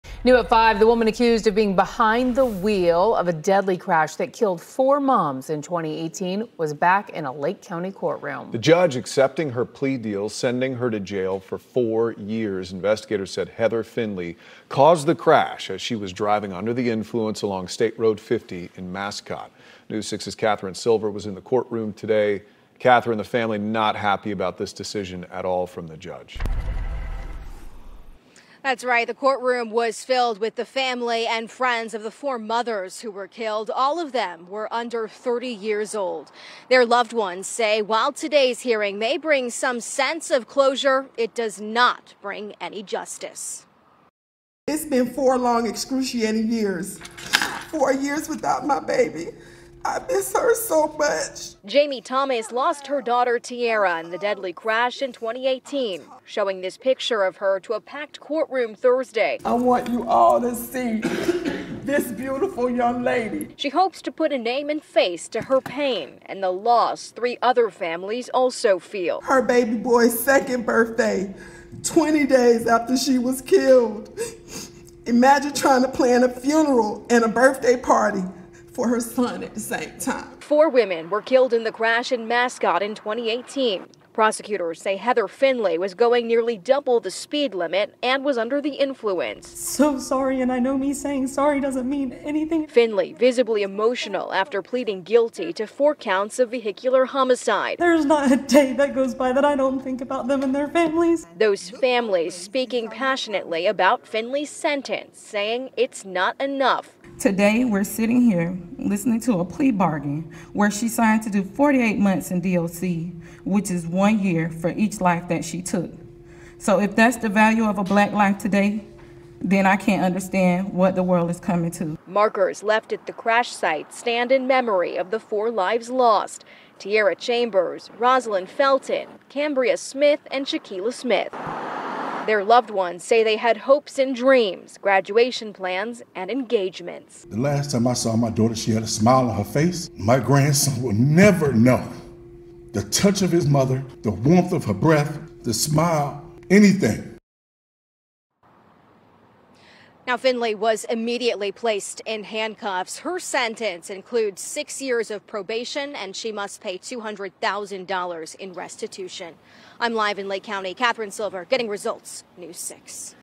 Judge accepts plea deal following deadly Lake County crash
A woman accused of being responsible for a deadly Lake County crash that  killed four women in 2018 had her plea deal accepted by a judge Thursday.